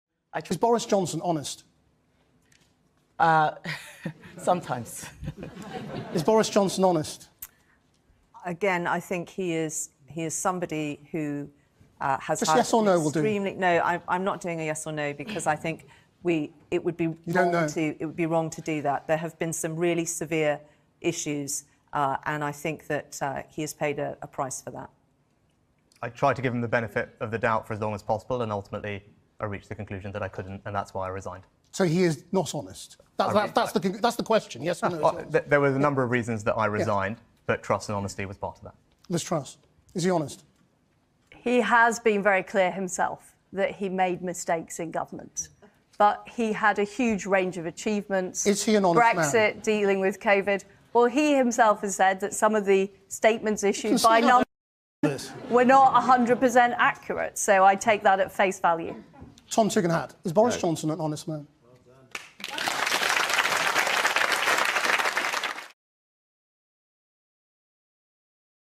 Tom Tugendhat shakes head when asked if Boris Johnson is honest
Candidates for prime minister and leader of the Conservative party were asked if they thought Boris Johnson was honest during the leadership debate on Channel 4 on Friday (15 July).

While Penny Mordaunt, Rishi Sunak, Kemi Badenoch, and Liz Truss gave longer responses, Tom Tugendhat shook his head.

The candidates' performance in the debate on Friday could influence the way Conservative MPs vote in the next leadership ballot on Monday (18 July).